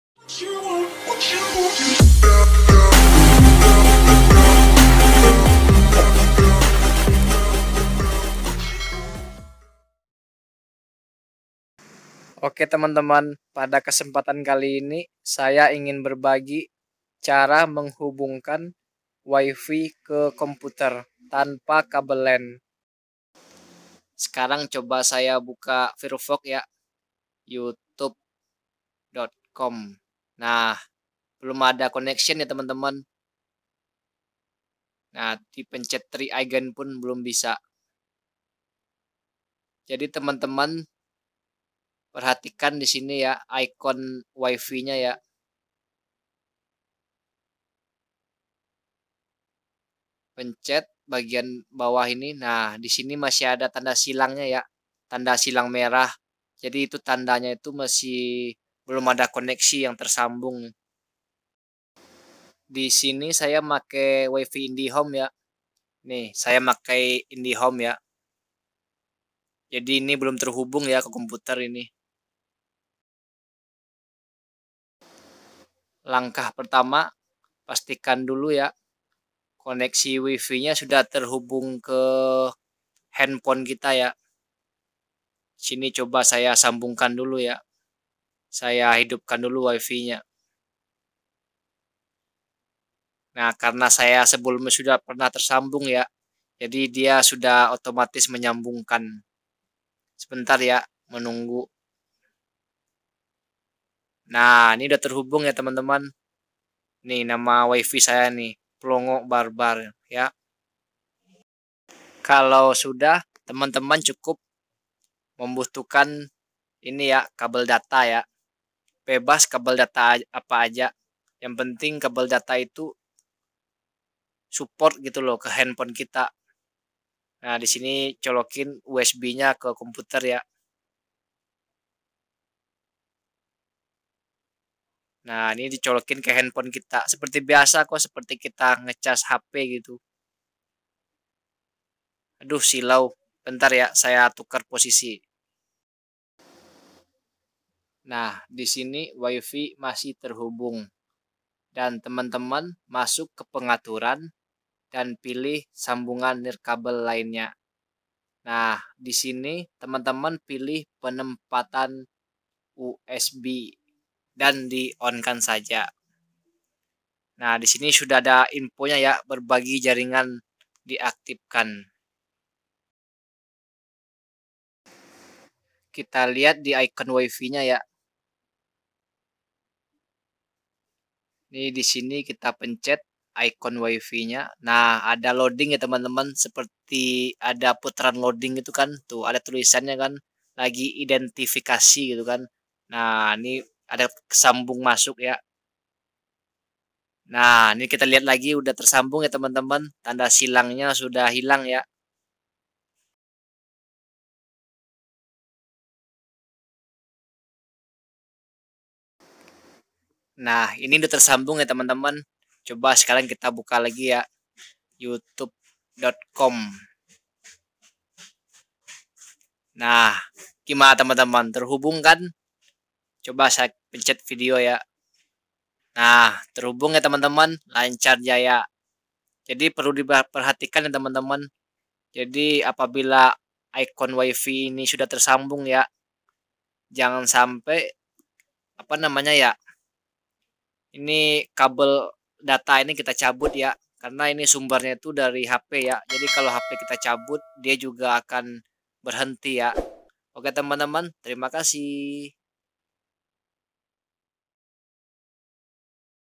Cara Menghubungkan Wifi ke Komputer tanpa Kabel lan
Cara untuk menghubungkan Wifi ke komputer tanpa Kabel,caranya sangat mudah
ini adalah salah satu solusi bagi komputer yang tidak mau terhubung dengan Wifi
banyak penyebab nya ,dan pada kesempatan kali ini saya akan memberikan tutorial nya,
Gimana sih cara nya ??
Tonton video ini sampai selesai yah.